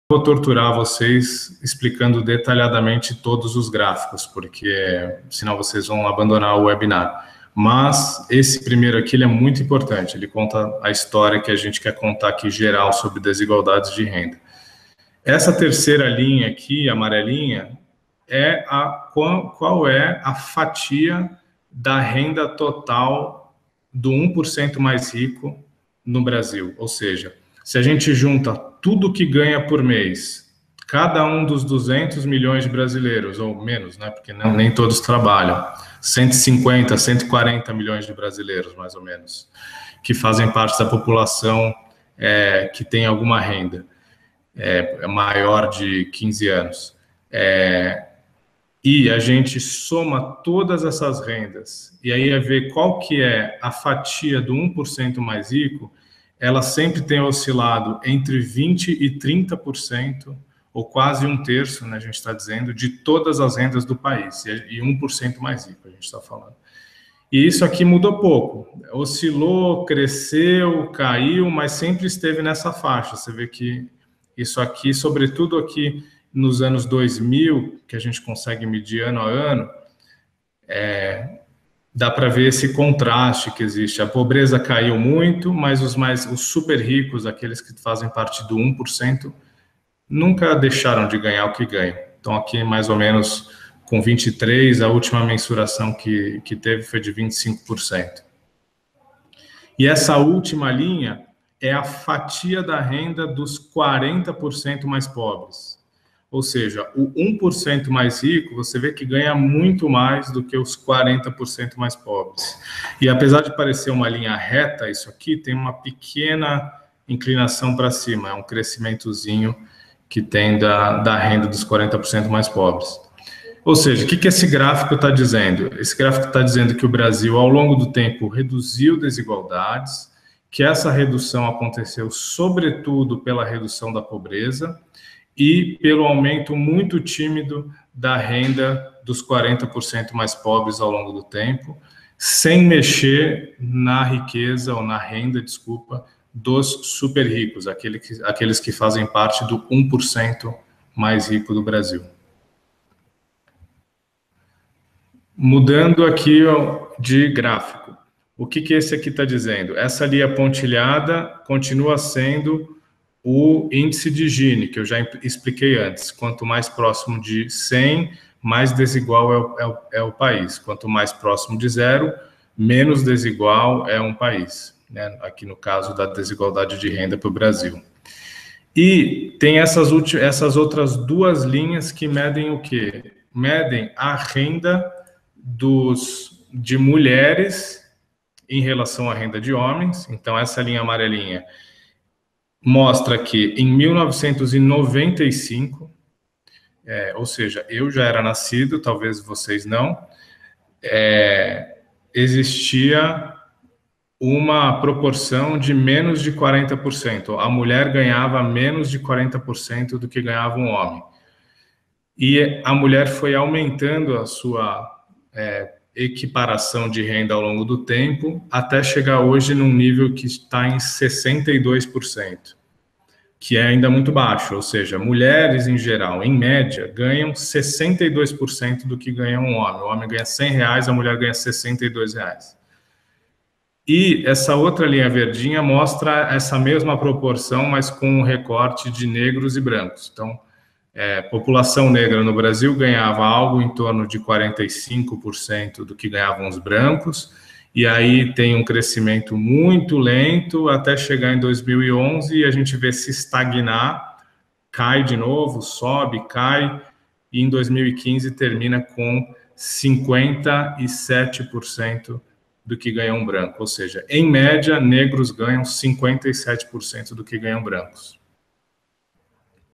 Webinar DesigualdadesNoEnem com Rafael Georges - Parte 2
O Coordenador de Campanhas da Oxfam Brasil, Rafael Georges, faz uma conversa sobre desigualdades, apresentando dados e análises sobre a situação brasileira, especialmente para quem está prestando Enem e Vestibular. Aproveite!